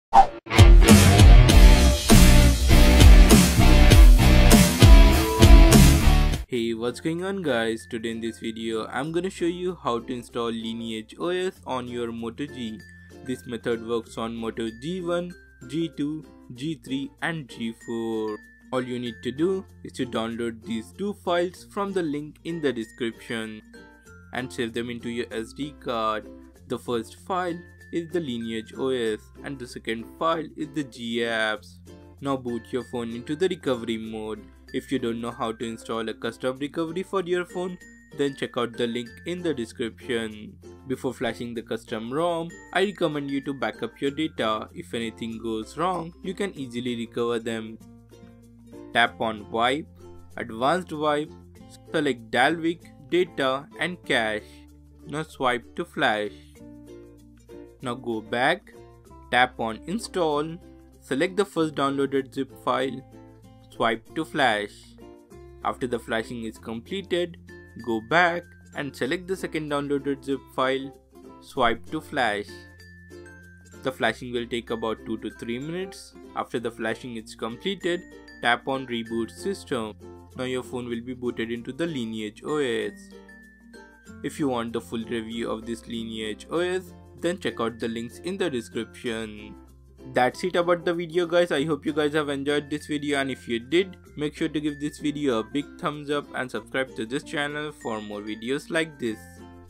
Install Lineage OS on Moto G(G1, G2, G3 and G4)
Android Nougat 7.1 is the latest update from Google.  Many Users are facing the issue to get the latest android n 7.1 on their Moto G. So, I have made a combined video to get Android N 7.1.1 on Moto G(1st Gen,2nd Gen, 3rd Gen and 4th Gen).This is the easiest method to install Android N 7.1 on Your Moto G(1st Gen,2nd Gen, 3rd Gen and 4th Gen). This is not the official update of Android N. This is the Nighty Build Version of Android N 7.1 which is based on Lineage OS. Lineage OS is the replacement of CyanogenMod. Previously, I made a video to get CM 14.1 based on Android N 7.1 on Moto G(1st Gen,2nd Gen, 3rd Gen and 4th Gen).As CM 14.1 is now outdated, you can install Lineage OS on your Moto G.. There are many methods to install this Android Nougat 7.1(Lineage OS) on your Moto G. I have shown the combined method to update Android N 7.1 Unofficial on Your Moto G(g1,g2,g3,g4/g4plus). The only thing you need to do is to download the two files of your device from below which are Lineage OS(Android N 7.1) Zip file and G Apps. Flash Android N 7.1 Zip file on Your Moto G and then flash G apps and you are done.

Downloads:
For Moto G4(2016):

My Gear:
New Camera
Old Camera